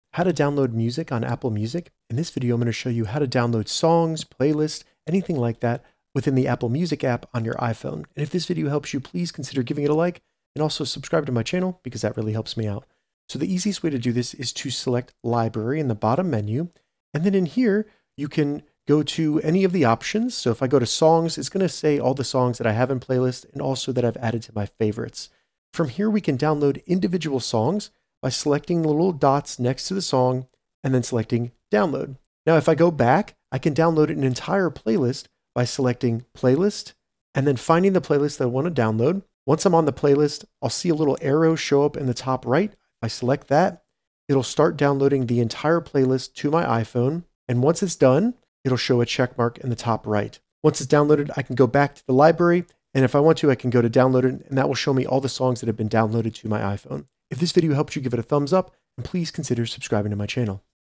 How to Download Music on Apple Music
How to download music on Apple Music quickly and easily. In this tutorial, I’ll show you step by step how to save songs, albums, and playlists for offline listening on your iPhone, iPad, or other devices using the Apple Music app. This means you can listen to your favorite tracks anytime without using data or Wi-Fi.